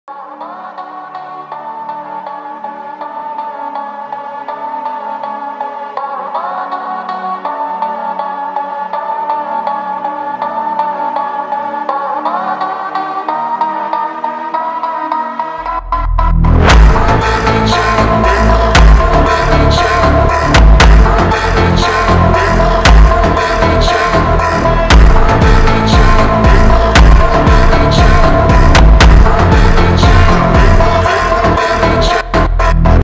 Jesus Areso🚀🔥.
jesus areso scored perfect goal
this goal can win the puskas award